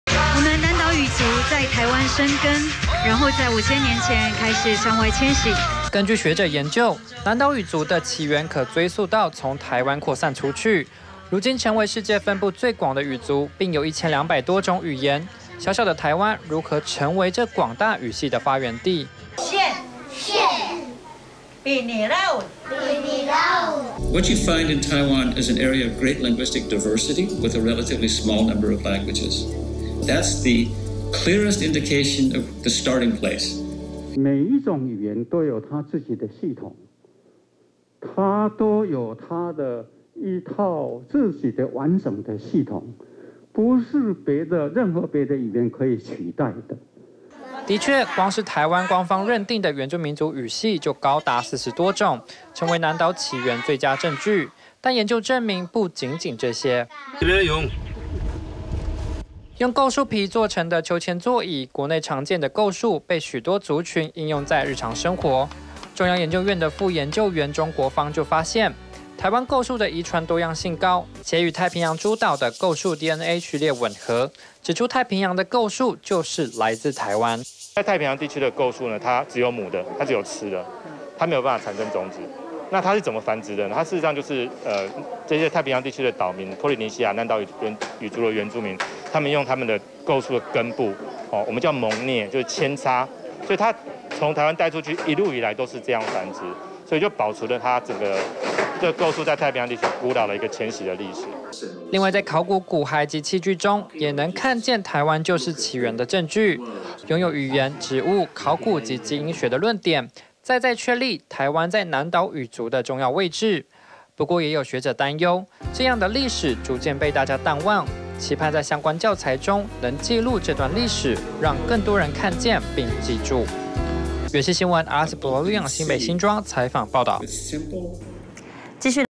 南島民族文化起源 研究發現台灣為關鍵角色 2021-08-17 IPCF-TITV 原文會 原視新聞
根據學者研究，南島語族的起源可追溯到從台灣擴散出去，如今成為世界分布最廣的語族，並有1,200多種語言，小小的台灣如何成為這廣大語系的發源地？

的確光是台灣官方認定的原住民族語系，就高達40多種，成為南島起源最佳證據。但研究證明不僅僅這些。

用構樹皮做成的鞦韆座椅，國內常見的構樹被許多族群應用在日常生活。中央研究院的副研究員鍾國芳就發現，台灣構樹的遺傳多樣性高，且與太平洋諸島的構樹DNA序列吻合，指出太平洋的構樹，就是來自台灣。

另外在考古骨骸及器具中，也能看見台灣就是起源的證據。擁有語言、植物、考古及基因學的論點，再再確立台灣在南島語族的重要位置。不過也有學者擔憂，這樣的歷史逐漸被大家淡忘，期盼在相關教材中能記錄這段歷史，讓更多人看見並記住。